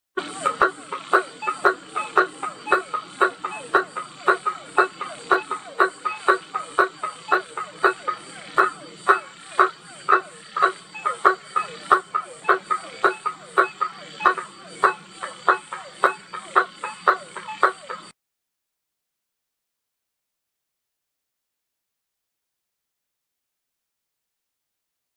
Sapo-martelo cantando Jd Botânico SP 4 12 2019 Antonio Silveira
Frogs of Brazil by Antonio Silveira
Bpana faber